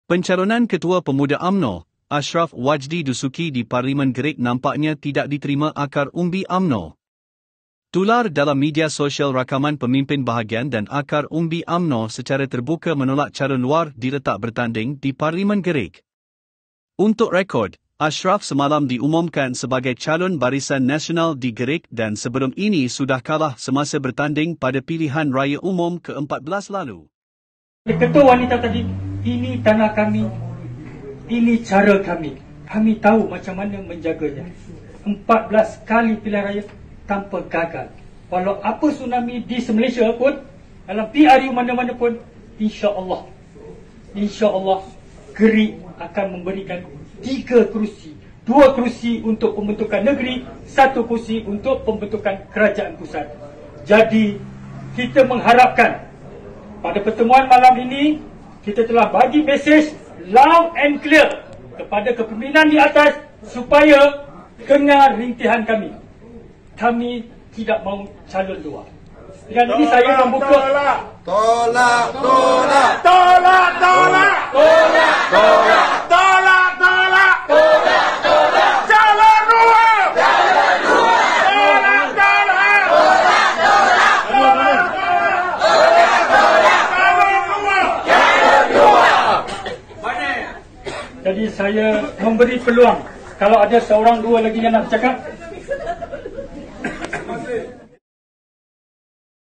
Rakaman akar umbi tolak Asyraf Wajdi jadi calon Gerik tular
Pencalonan Ketua Pemuda UMNO, Asyraf Wajdi Dusuki di Parlimen Gerik nampaknya tidak diterima akar umbi UMNO.

Untuk rekod, Asyraf semalam diumumkan sebagai calon BN di Gerik dan sebelum ini sudah kalah semasa bertanding pada Pilihan Raya Umum Ke-14 lalu.